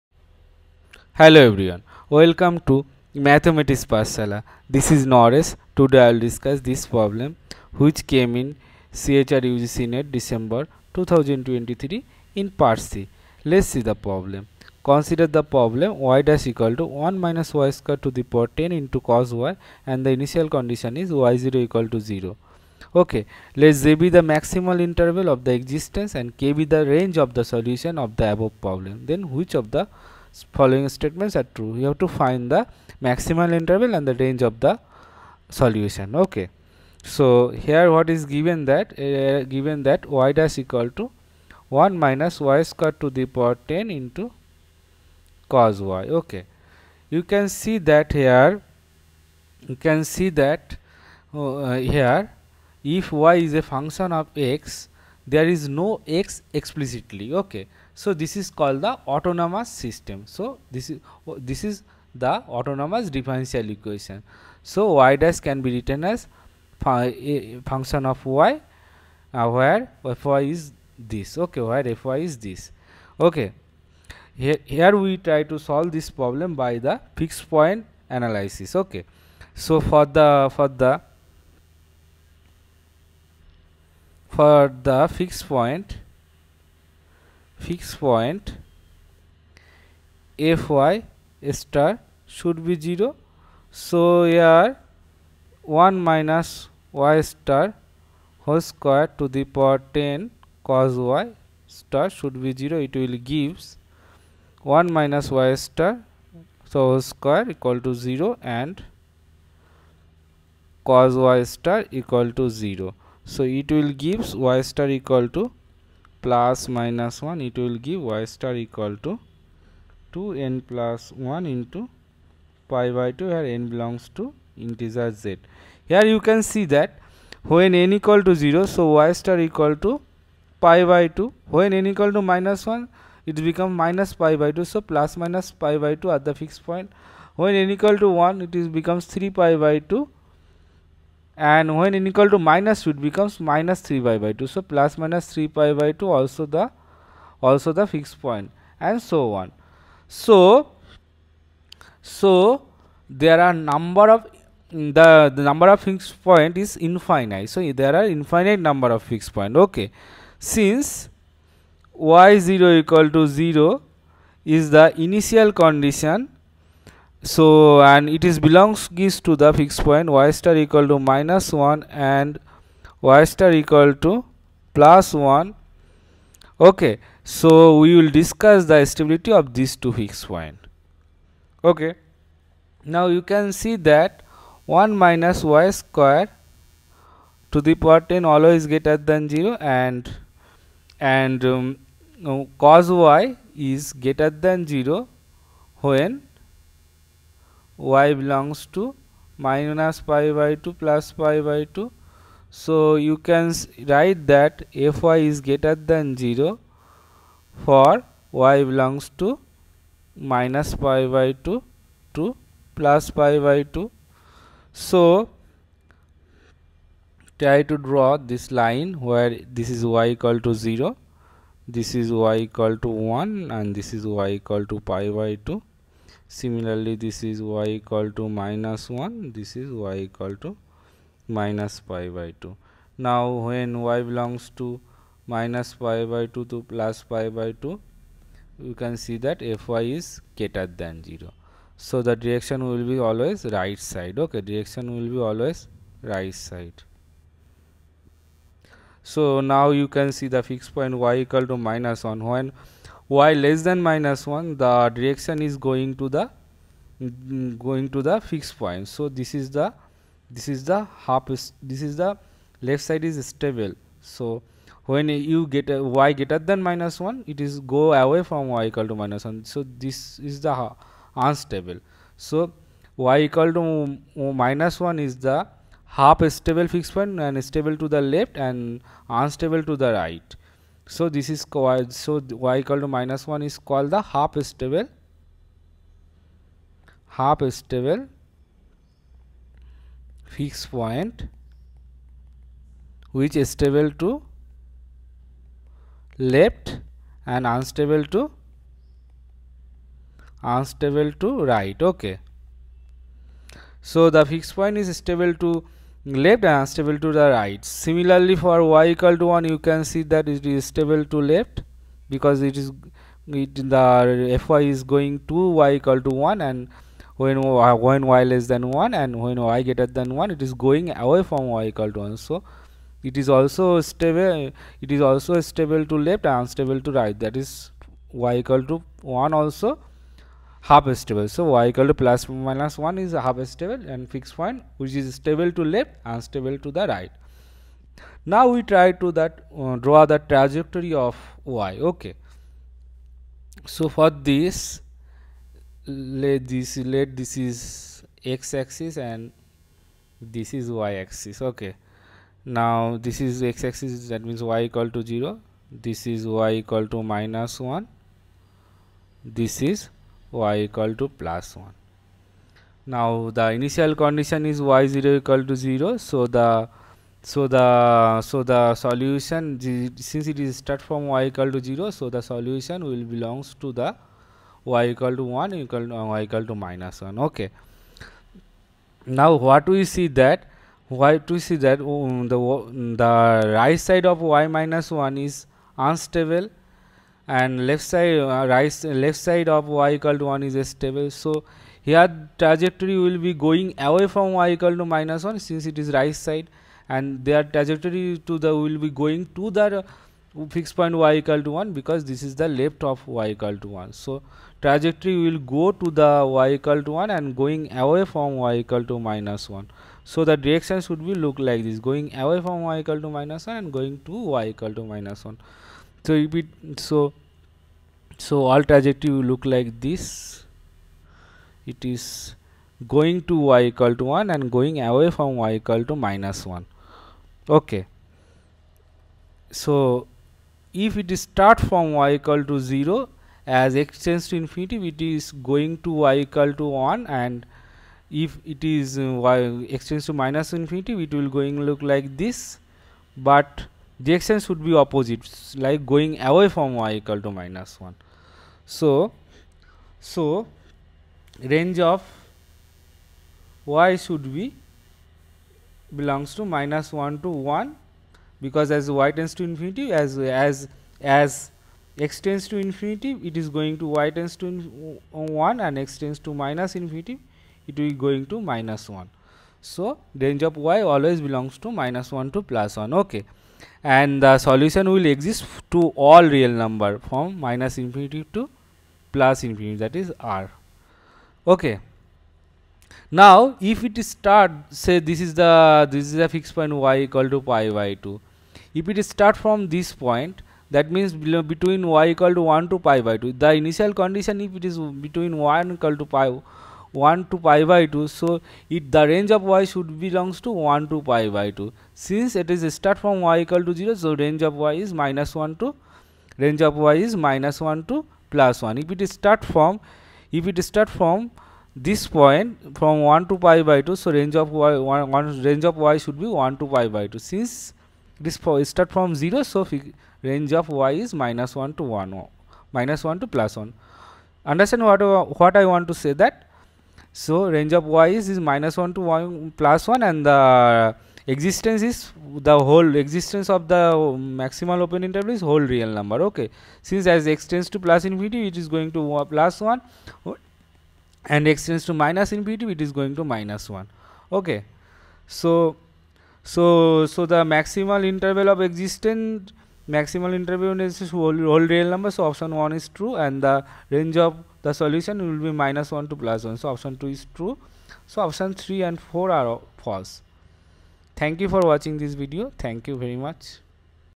CSIR UGC NET December 2023 | Mathematical Sciences | ODE | Part C | Questions ID 704091
CSIR NET December 2023, Ordinary differential equations Solutions,
Mathematical Science
Part-C Question, CSIR NET December 2023 Exam.
Question ID 704091.